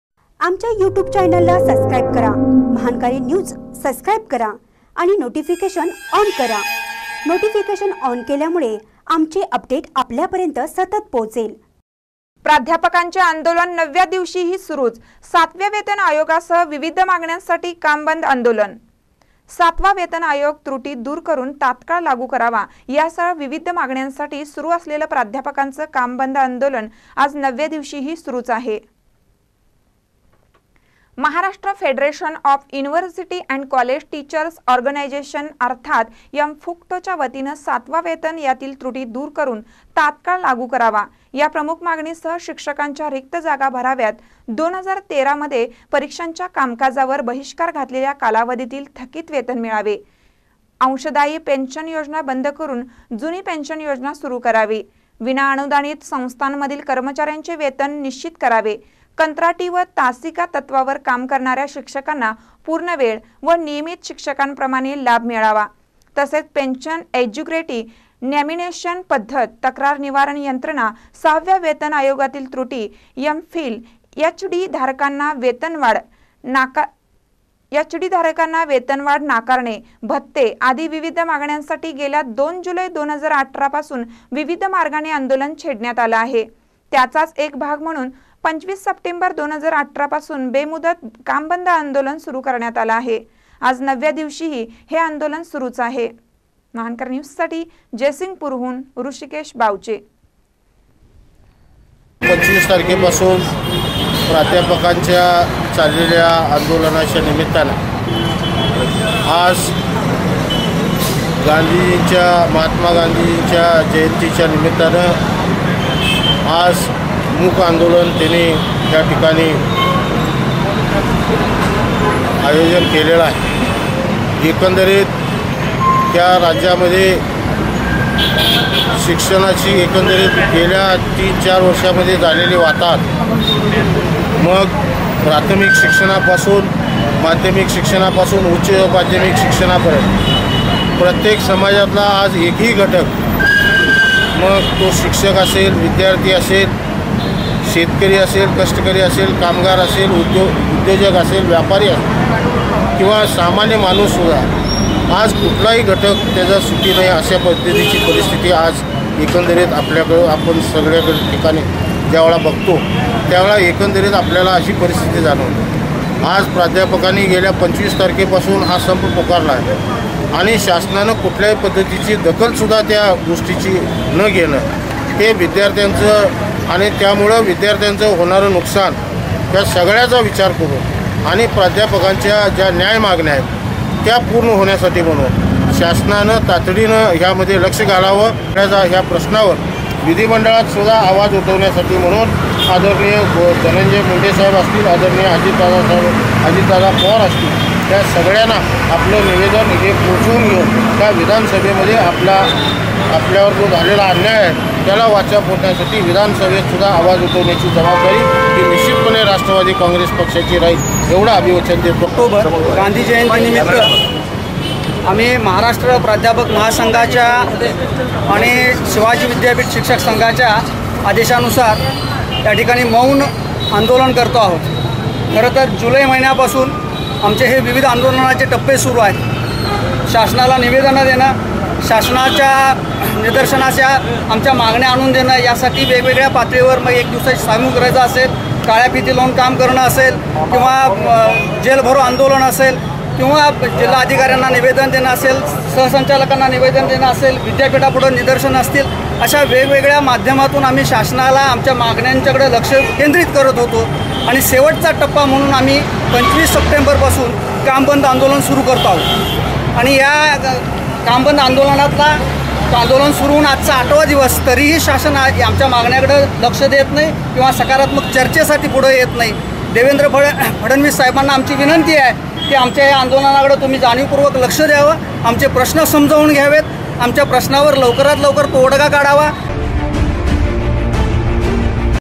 प्राध्यापकांचे आंदोलन नवव्या दिवशीही सुरुच : सातव्या वेतन आयोगासह विविध
प्राध्यापकांचे आंदोलन नवव्या दिवशीही सुरुच : सातव्या वेतन आयोगासह विविध मागण्यांसाठी काम बंद आंदोलन